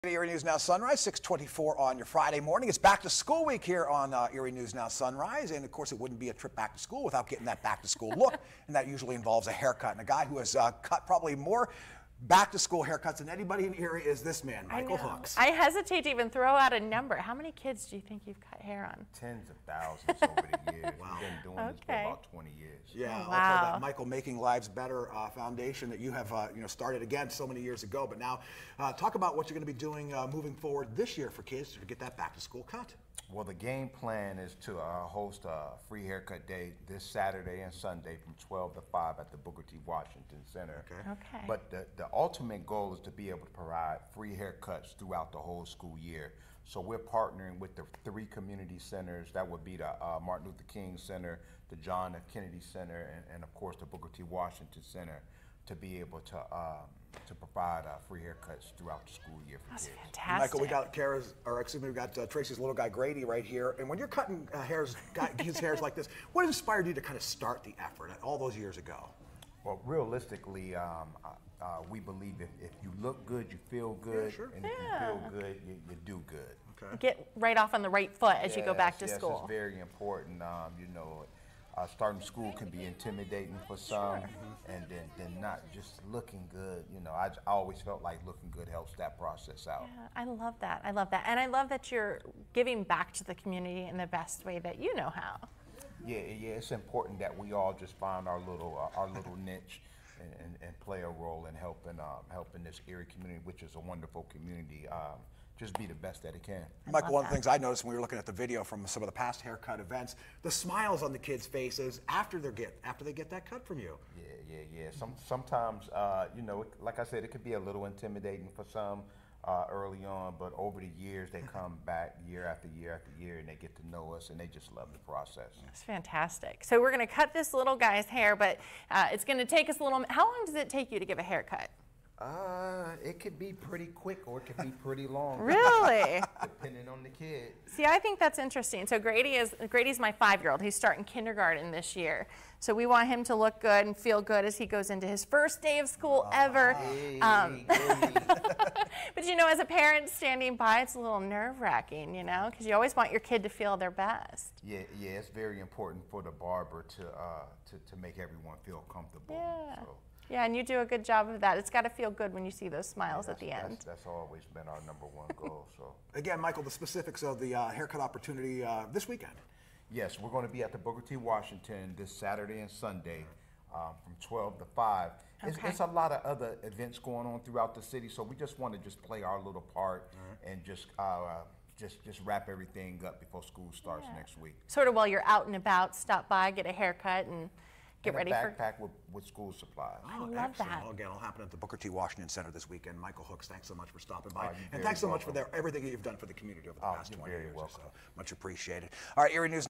Free Back-to-School Haircuts
Mike Hooks discusses free back-to-school haircuts.